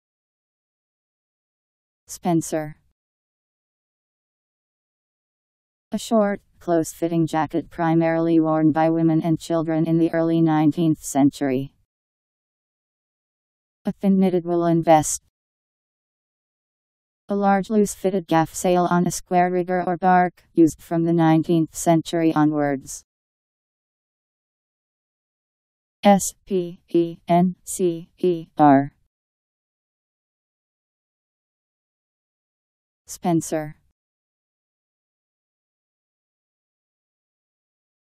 Spencer Meaning
Video shows what spencer means. A short, close-fitting jacket primarily worn by women and children in the early nineteenth century.. A thin knitted woollen vest.. A large loose-fitted gaffsail on a square-rigger or barque, used from the nineteenth century onwards..  Spencer Meaning. How to pronounce, definition audio dictionary. How to say spencer. Powered by MaryTTS, Wiktionary